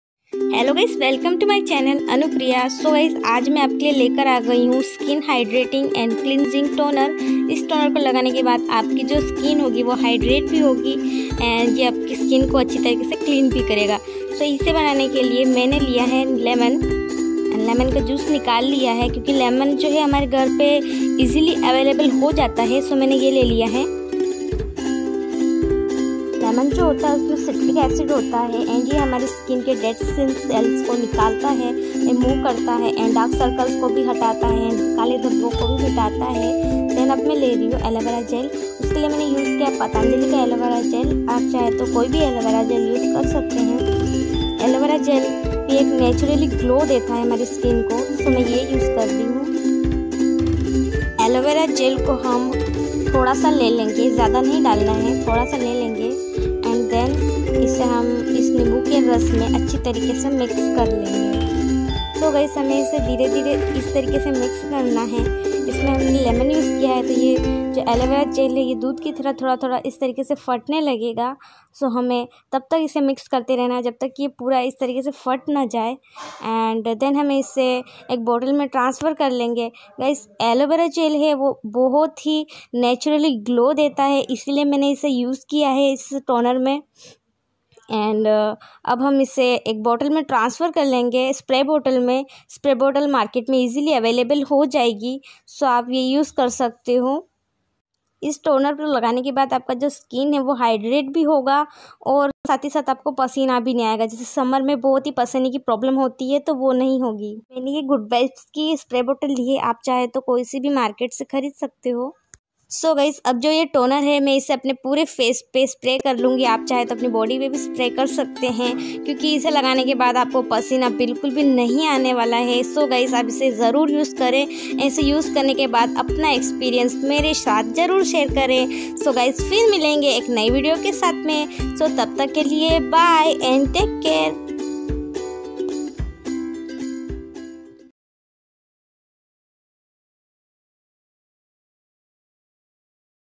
FACE TONER FOR SUMMER|TONER FOR OILY SKIN| SKIN WHITENING|घर पर स्किन टोनर बनाने का तरीका
HELLO FREINDS,
ANUPRIYA.

Today i will show you how to make face toner at home for summer oily skin.ingridients which i use in this toner is totally natural and easyly avilable at home.

1TBSP-LEMON JUICE
1TBSP-ALOVERA GEL

TONER FOR SUMMER,TONER FOR OILY SKIN,HOW TO MAKE A FACIAL TONER AT HOME,SKIN TONER,MAGICAL TONER.ACNE PRONE SKIN TONER.

आज में आपको समर/गर्मी फेस टोनर बनाना बतौंगी इसे बनाने के लिए आपको जो चीज़े चाहिए वो आसानी से अपक घर में मिल जाएगी और ये सभी प्राक्रतिक हे जो इस टोनर को बनाने में उपयोग होगी|